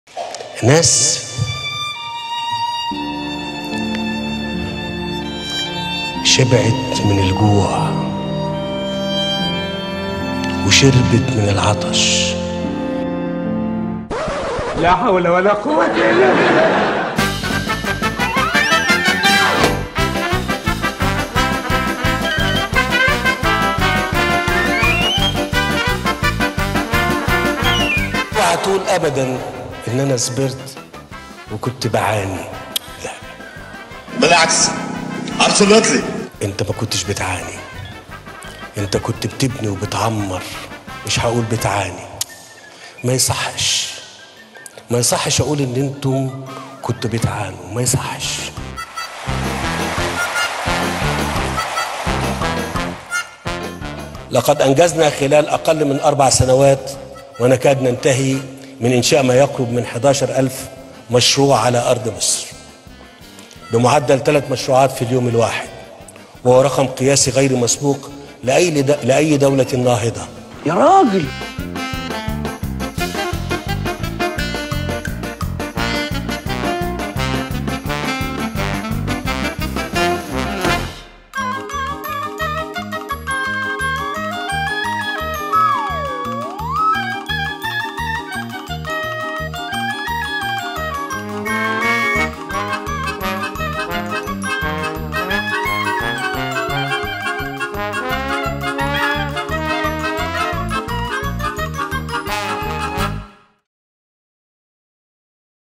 "حكاية وطن" السيسي
السيسي يستعرض ما حققه خلال فترته الرئاسية في مؤتمر  "حكاية وطن" وسخرية على مواقع التواصل